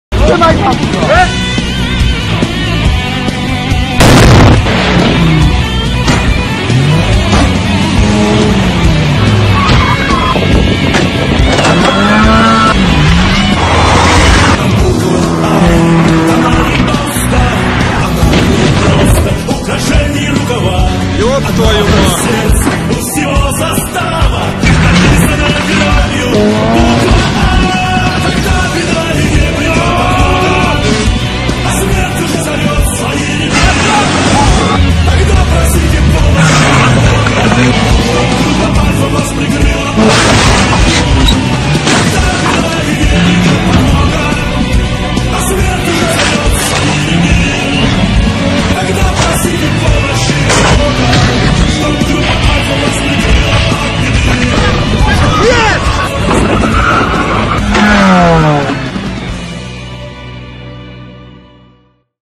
ALPHA GROUP SPECIAL VEHICLE ASSAULT SQUADRON
Heroic and strenuous training regime of the vehicle assault squadron allegedly responsible for 47% of incidents caught on dashcam in Russia